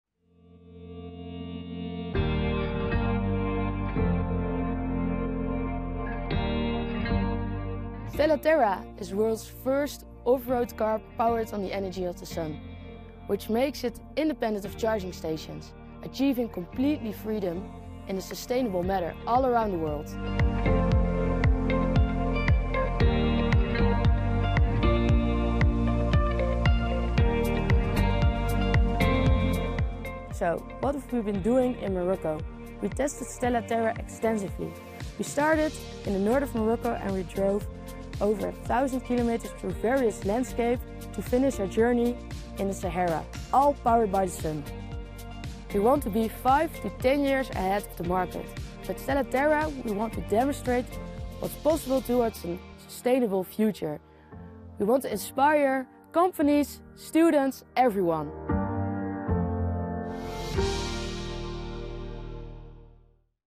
Dutch students test-drive the world’s first off-road solar vehicle in Morocco
The two-seater, road-legal ‘Stella Terra’ model is powered via solar roof panels and has a 630 km range on a sunny day.